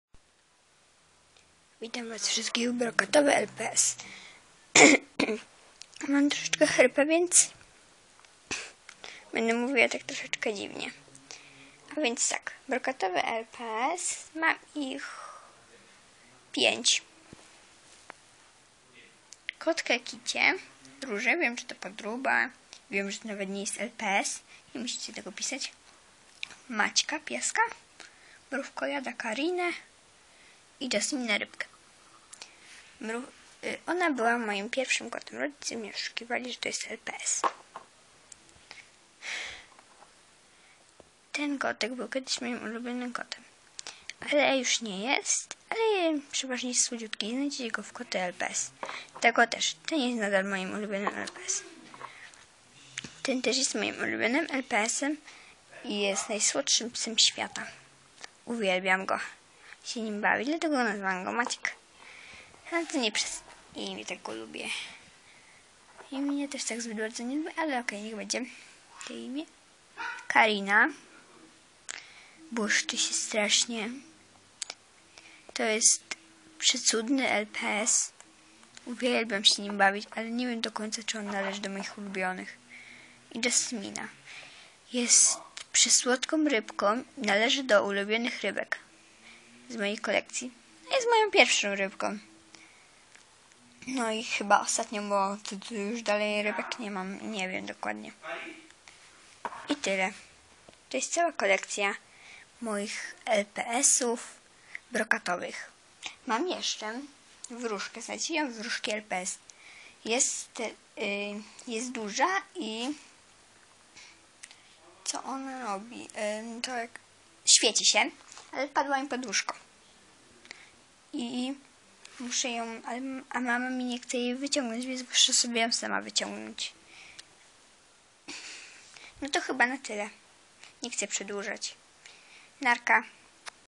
Brokatowe lps #7
Ta wróżka też jest brokatowa :) a Oto kolekcja brokatowych LPS :) jest ich dokładnie 6 a nie 5 :)